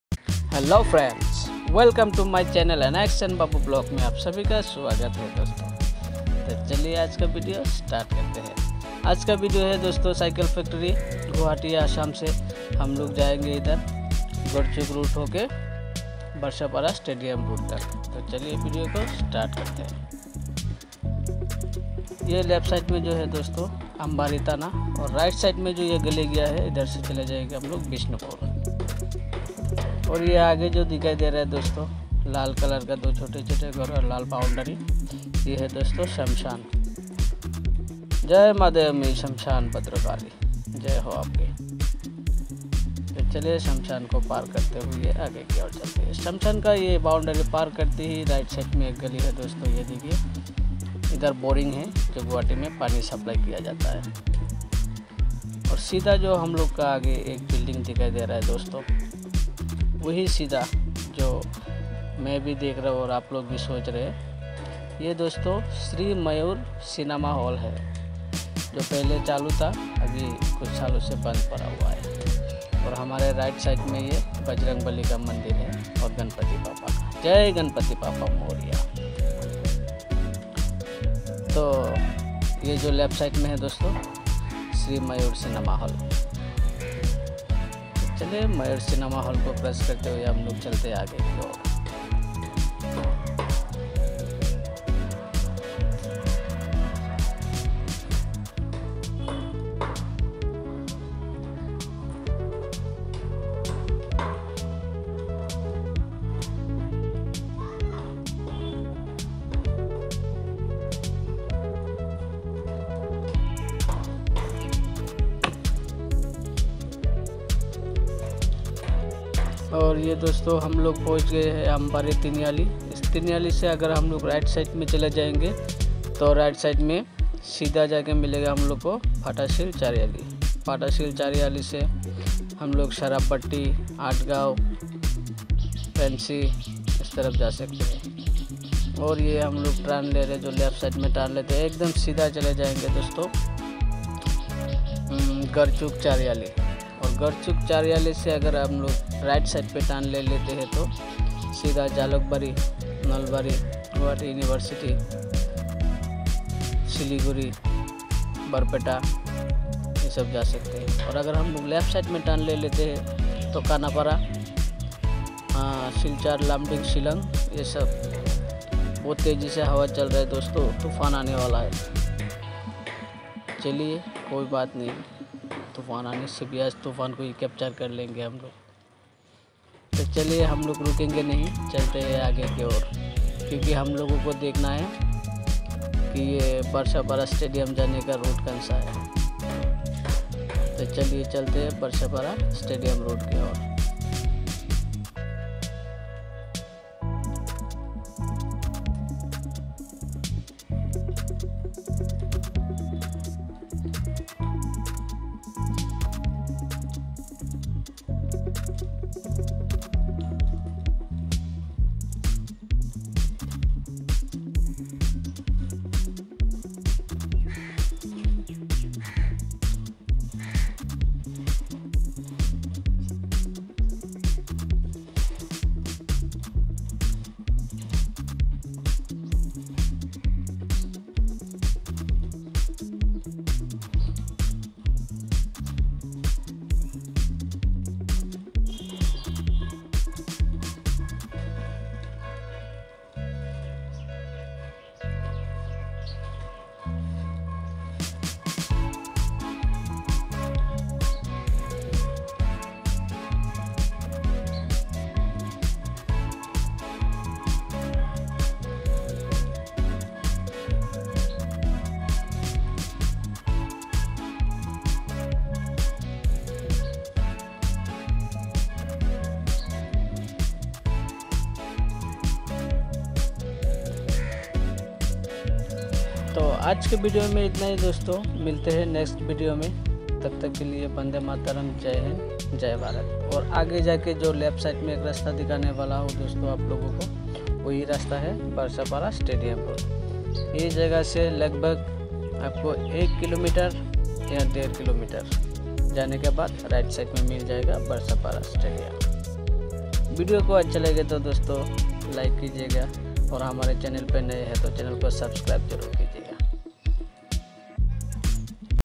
Cycle factory to Barsapara Cricket Stadium road | guwahati cycle factory
coverage topic

1)cycle factory
2)ambari tana
3)ambari shoshan
4)bishnupur road
5)fatashil road
6)garchuk Road